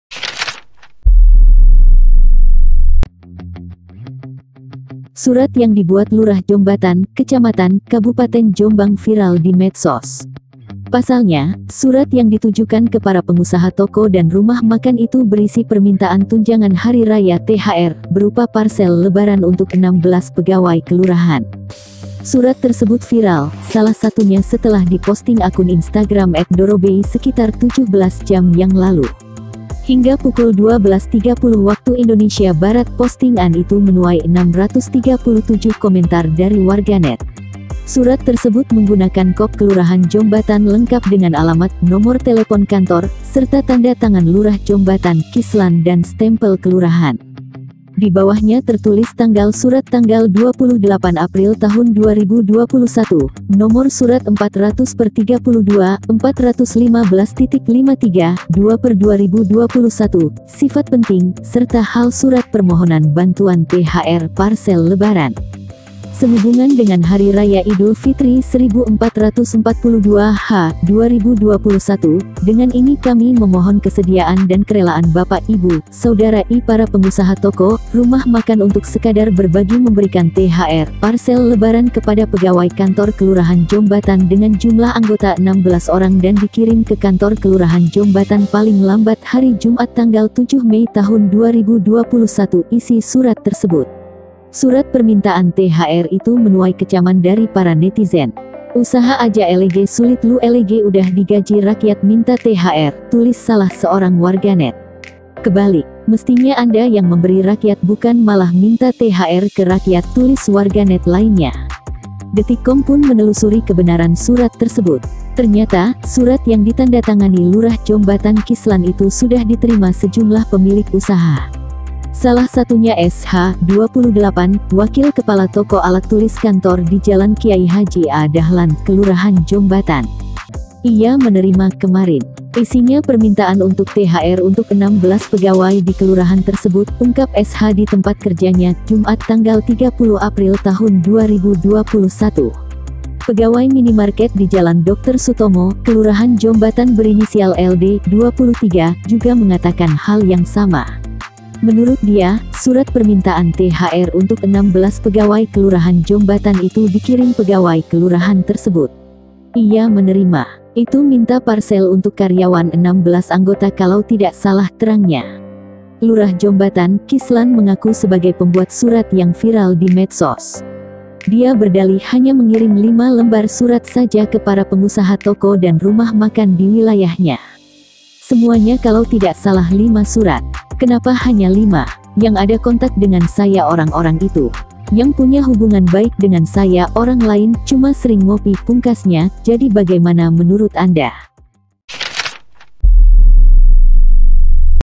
di Jombang ada Lurah Minta THR ke Pengusaha Toko dan Rumah Makan"[BERITA TERKINI]"
GITU-DOANG di Jombang ada Lurah Minta THR ke Pengusaha Toko dan Rumah Makan"[BERITA TERKINI]"